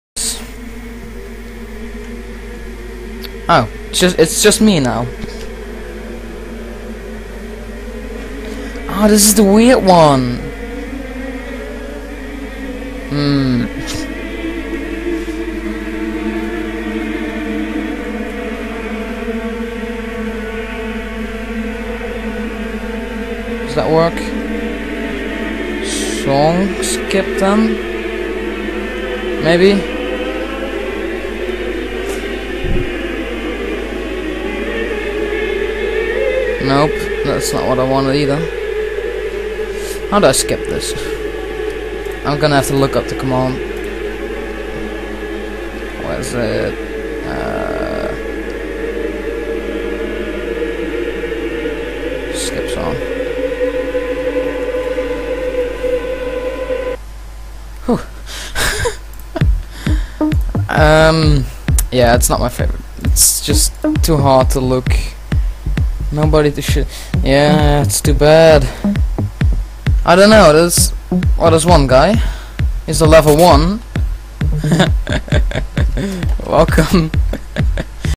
Oh this is the weird one!
I was trolling my friend again while streaming... It was quite funny I guess!